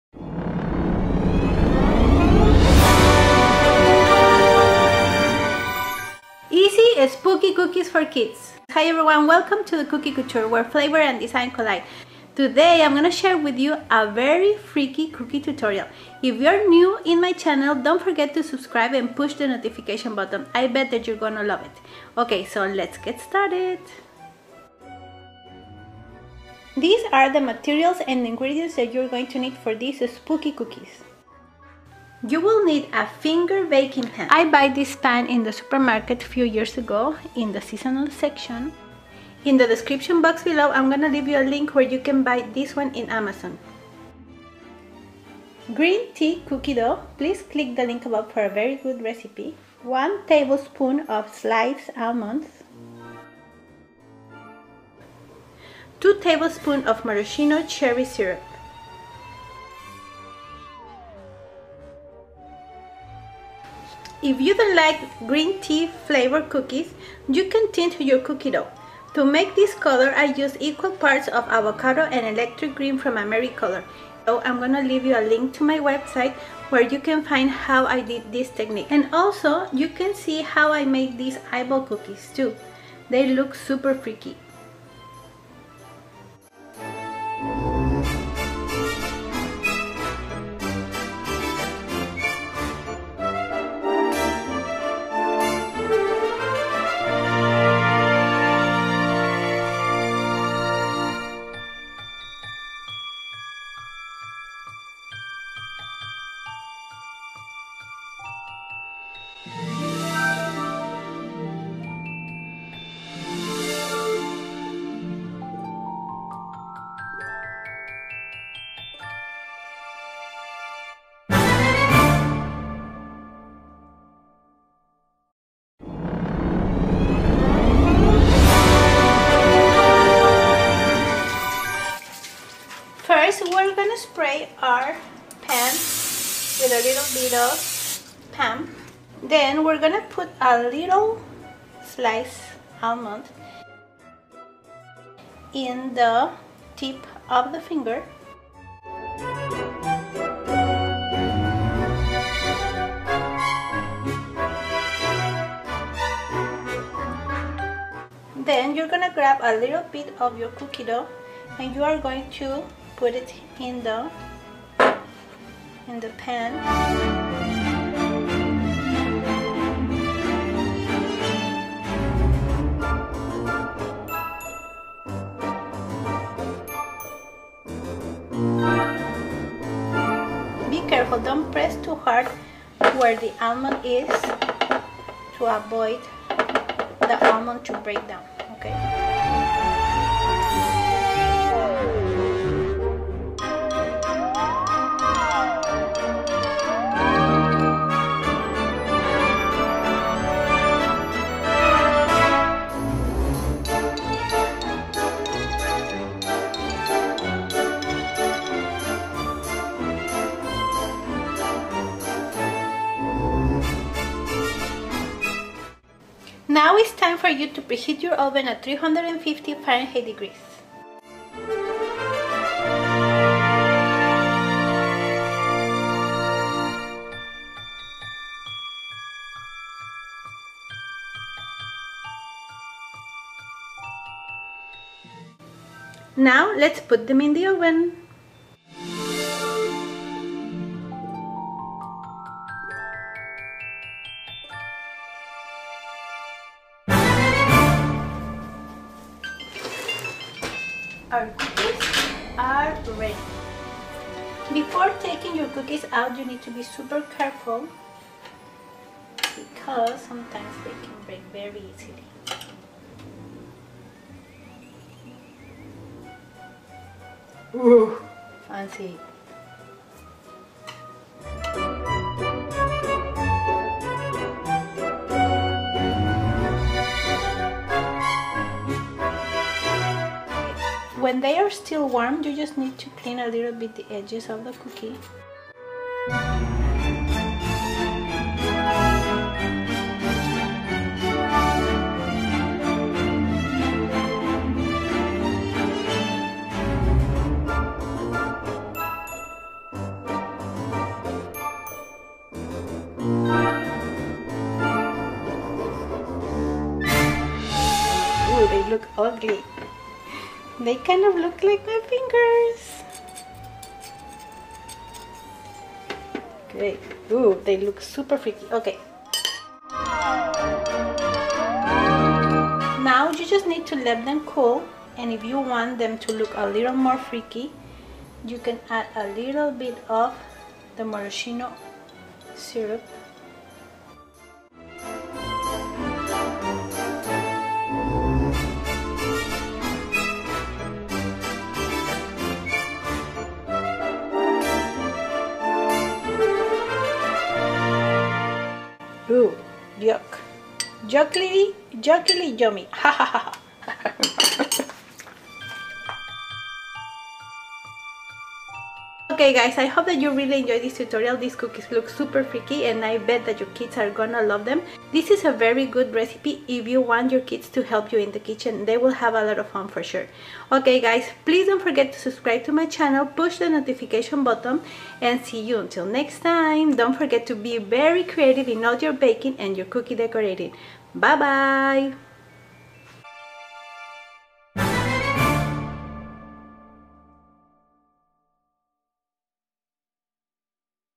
Easy Spooky cookies for kids
You will see how to make spooky fingers cookies, two different ways. The kids will love to put them self in action baking cookies for halloween.

Finger Baking Pan

Meringue Powder

Dark chocolate

Baking sheets